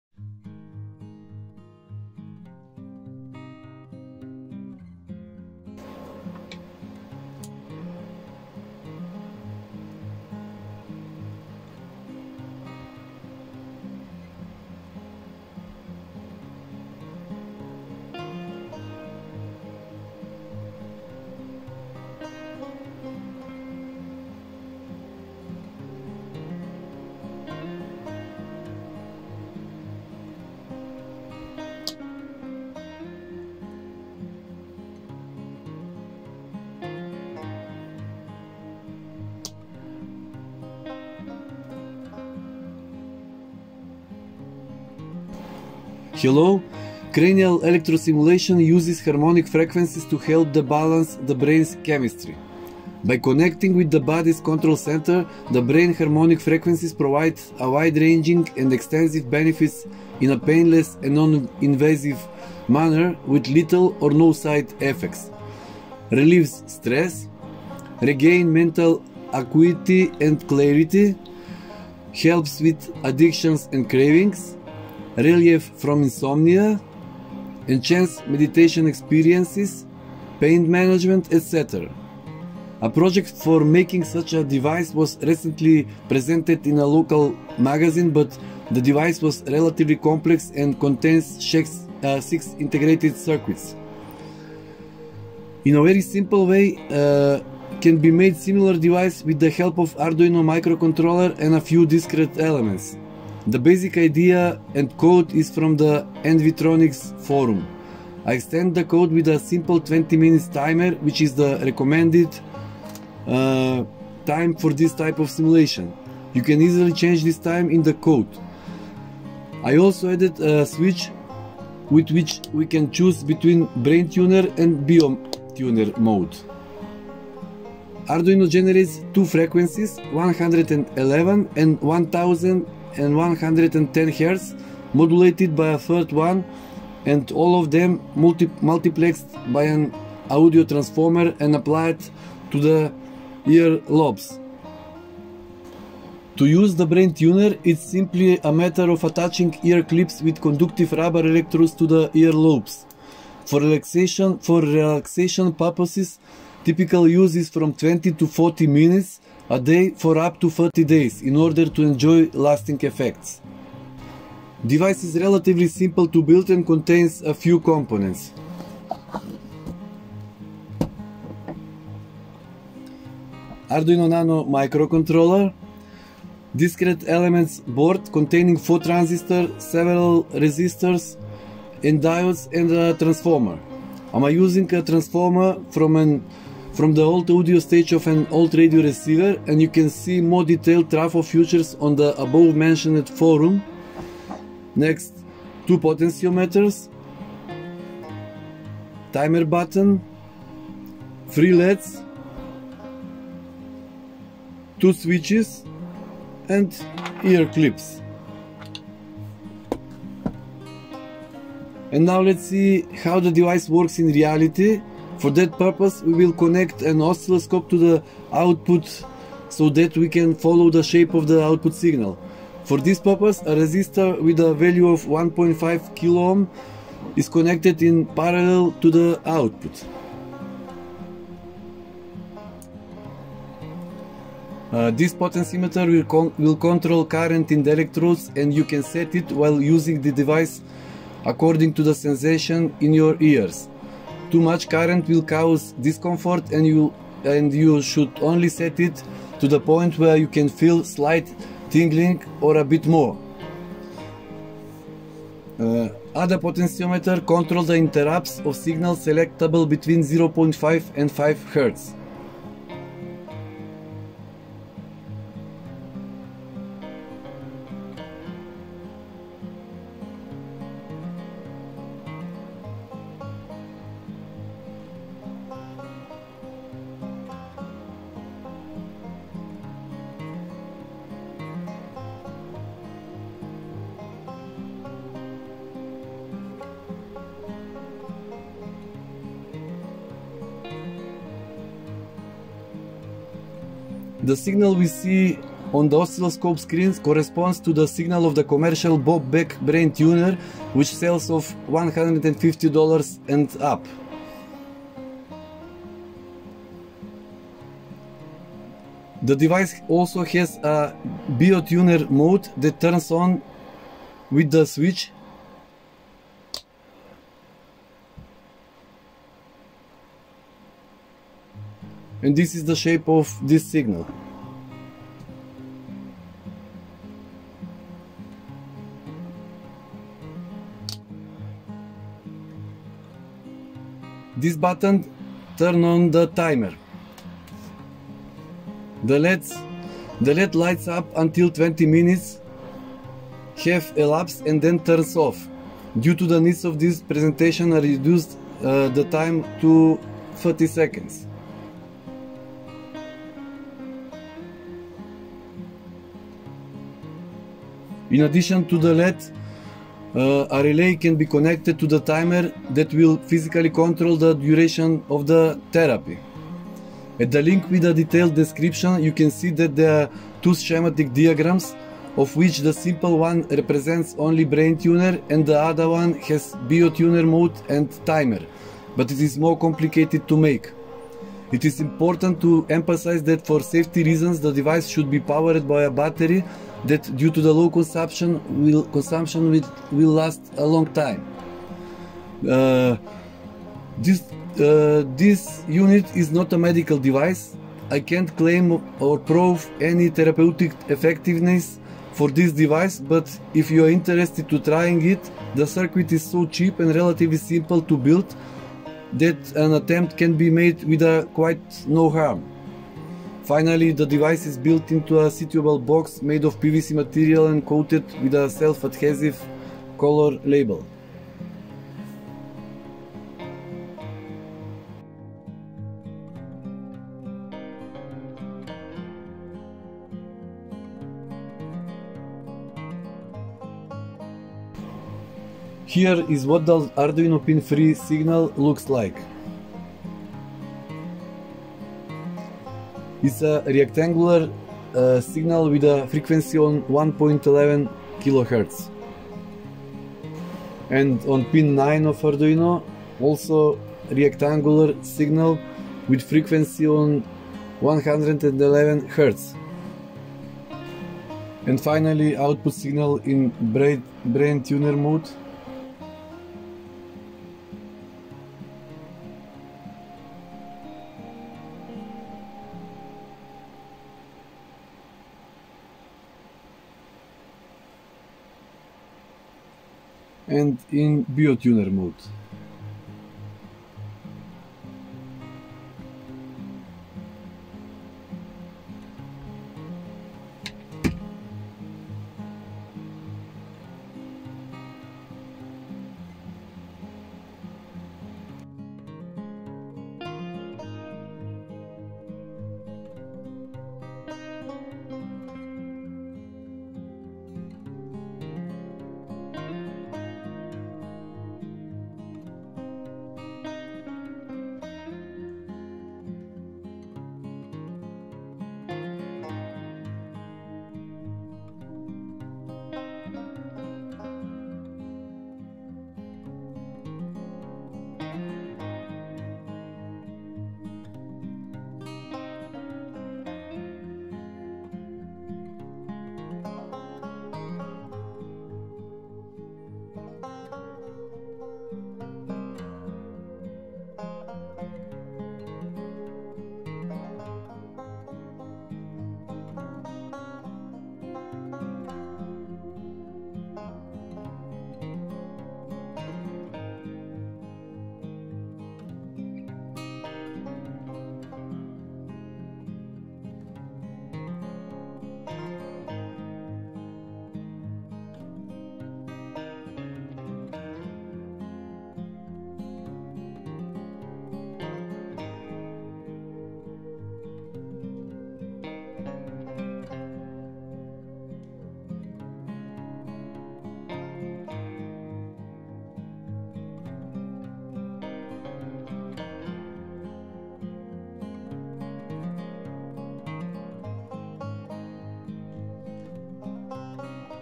Arduino Cranial Stimulator Bio BrainTuner
Cranial electro stimulation uses harmonic frequencies to help balance the brain's chemistry. By connecting with the body's control center, the brain harmonic frequencies provide wide-ranging and extensive benefits in a painless and non-invasive manner, with little or no side effects:
   - Relieves stress
   - Regain mental acuity and clarity
   - Helps with addictions & cravings
   - Relief from insomnia
   - Enhance meditation experiences
   - Pain management   etc...
  A project for making such a device was recently presented in a local magazine, but the device was relatively complex and contained 6 Integrated Circuits. In a very simple way can be made similar device with the help of Arduino microcontroller and a few discrete elements. The basic idea and code is from the nvtronics.org forum. I extended the code with a simple 20 min timer, which is the recommended time for this type of simulation. You can easily change this time in the code in line:
timerCount = 20 * 60 * 10; (20 mins * 60 sec * 10 * 1 deciseconds)
   I also added a switch with which we can choose between Brain Tuner and Bio Tuner mode. Arduino generates two frequencies(111 Hz and 1110 Hz) modulated by a third one and all of them multiplexed by an audio transformer and applied to ear lobes.
   Device is relatively simple to build and contains a few component:
       - Arduino Nano microcontroller
       - Discrete element board containing 4 transistors, several resistors and diodes, and transformer
       - I am using a transformer from the output audio stage of an old radio receiver, and you can see more detailed trafo features on the above mentioned forum
       - Two potentiometers
       - Button
       - two leds
       - and Ears Clips
   And now let's see how the device works in reality. For that purpose, we will connect an oscilloscope to the output so that we can follow the shape of the output signal. For this purpose, a resistor with a value of 1.5 kiloohm is connected in parallel to the output. First potentiometer will control current in electrodes & you can set it  while using device according to sensation in your ears. Too much current will cause discomfort, and you should only set it to the point where you can feel slight tingling or a bit more. Other potentiometer control the interrups of signal selectable between 0.5 and 5Hz. The signal we see on the oscilloscope screen corresponds to the signal of the commercial Bob Beck Brain Tuner which sells for $ 150 and up. Тhe device also has a Bio Tuner mode that turns on with the switch, and we can the shape of this signal on oscilloscope.
   Yellow button turns on the timer. The LED lights up until 20 minutes have elapsed and then turns off. Due to the needs of this presentation, I reduced the time to 1 minute. In addition to the LED, a relay can be connected to the timer that will physically control the duration of the therapy. At the link with a detailed description you can see that there are two schematic diagrams, of which the simple one represents only Brain Tuner, and the other one has Bio tuner mode and timer, but it is more complicated to make. It is important to emphasize that for safety reasons the device should be powered by a battery that due to low consumption will last a long time
  This unit is not a medical device. I can’t claim or prove any therapeutic effectiveness for this device, but if you are interested in trying it, the circuit is so cheap and relatively simple to build that an attempt can be made with quite no harm.
  Finally, the device is built into a suitable box made of PVC material and coated with a self-adhesive color label.
If you are considering using this device for health-related purposes, it is crucial to consult a qualified healthcare professional to ensure safety and efficacy.
This video only explains the technical aspect of this device (how to build it) .

If you are interested in this area, see the Playlist with all my  simple PEMF devices at: